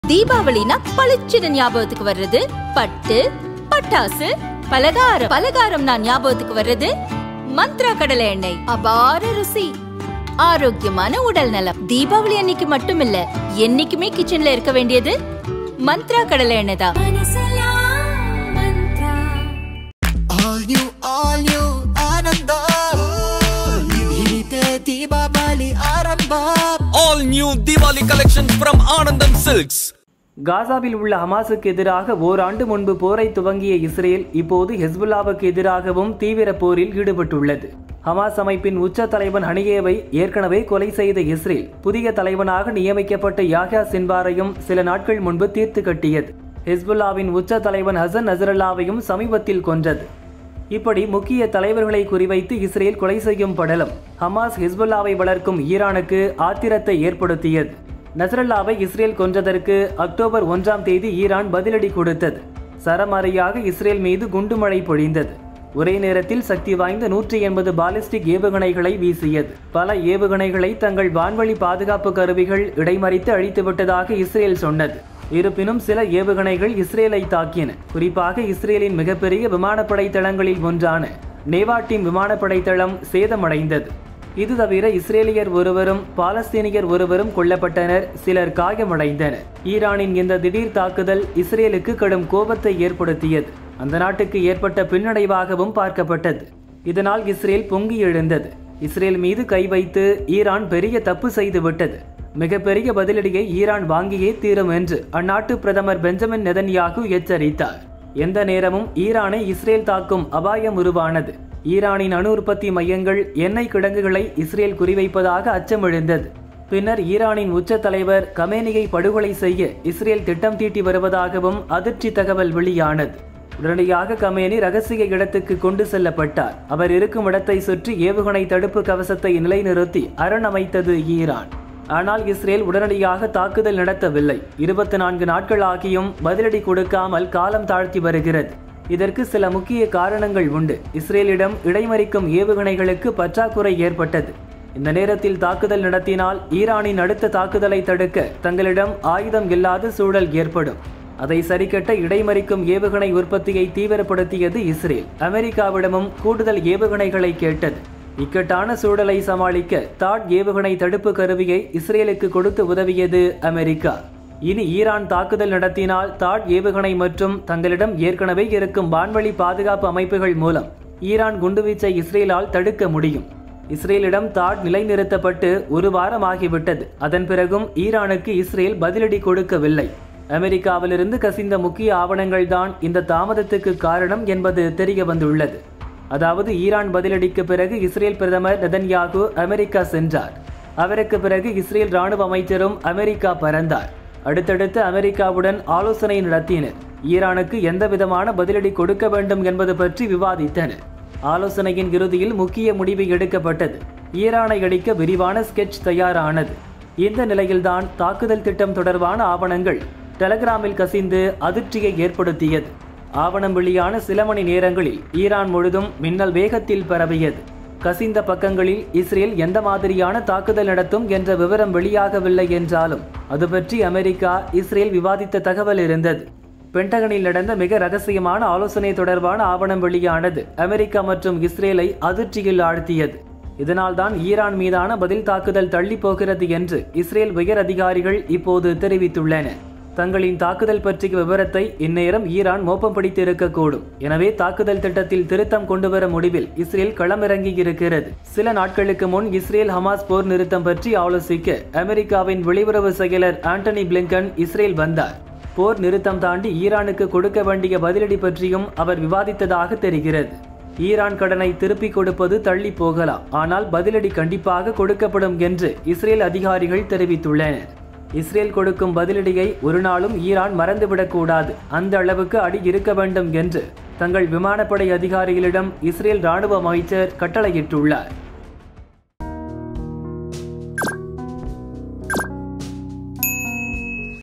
இஸ்ரேல் ஷாக்... ஈரான் பதிலடி திட்டத்தில் மெகா ஓட்டை Israel vs Iran | israel vs hezbollah | Pentagon
Partnership ஈரானுக்கு பதிலடி எப்போது?
இஸ்ரேல் திட்டத்தில் ஓட்டை!

கோட்டை விட்டது அமெரிக்கா

காசாவில் உள்ள ஹமாசுக்கு எதிராக ஓராண்டு முன்பு போரை துவங்கிய இஸ்ரேல் இப்போது ஹெஸ்புலாவுக்கு எதிராகவும் தீவிர போரில் ஈடுபட்டுள்ளது.

ஹமாஸ் அமைப்பின் உச்ச தலைவன் ஹனியேவை ஏற்கனவே கொலை செய்த இஸ்ரேல், புதிய தலைவனாக நியமிக்கப்பட்ட யாஹ்யா சின்வாரையும் சில நாட்கள் முன்பு தீர்த்துக்கட்டியது.

ஹெஸ்புலாவின் உச்ச தலைவன் ஹசன் நஸ்ரல்லாவையும் சமீபத்தில் கொன்றது.

இப்படி முக்கிய தலைவர்களை குறி வைத்து இஸ்ரேல் கொலை செய்யும் படலம், ஹமாஸ், ஹெஸ்புலாவை வளர்க்கும் ஈரானுக்கு ஆத்திரத்தை ஏற்படுத்தியது.

நஸ்ரல்லாவை இஸ்ரேல் கொன்றதற்கு அக்டோபர் 1ம் தேதி ஈரான் பதிலடி கொடுத்தது.

சரமாரியாக இஸ்ரேல் மீது குண்டு மழை பொழிந்தது. ஒரே நேரத்தில் சக்தி வாய்ந்த 180 பாலிஸ்டிக் ஏவுகணைகளை வீசியது.

பல ஏவுகணைகளை தங்கள் வான்வெளி பாதுகாப்பு கருவிகள் இடைமறித்து அழித்து விட்டதாக இஸ்ரேல் சொன்னது.

இருப்பினும் சில ஏவுகணைகள் இஸ்ரேலை தாக்கின. குறிப்பாக இஸ்ரேலின் மிகப்பெரிய விமானப்படை தளங்களில்  ஒன்றான நேவாடிம் (Nevatim) விமானப்படை தளம் சேதம் அடைந்தது.

இது தவிர இஸ்ரேலியர் ஒருவரும், பாலஸ்தீனியர் ஒருவரும் கொல்லப்பட்டனர். சிலர் காயம் அடைந்தனர்.

ஈரானின் இந்த திடீர் தாக்குதல் இஸ்ரேலுக்கு கடும் கோபத்தை ஏற்படுத்தியது. அந்த நாட்டுக்கு ஏற்பட்ட பின்னடைவாகவும் பார்க்கப்பட்டது.

இதனால் இஸ்ரேல் பொங்கி எழுந்தது. &#39;இஸ்ரேல் மீது கை வைத்து ஈரான் பெரிய தப்பு செய்து விட்டது. மிகப்பெரிய பதிலடியை ஈரான் வாங்கியே தீரும்&#39; என்று அந்நாட்டு பிரதமர் பெஞ்சமின் நெதன்யாகு எச்சரித்தார்.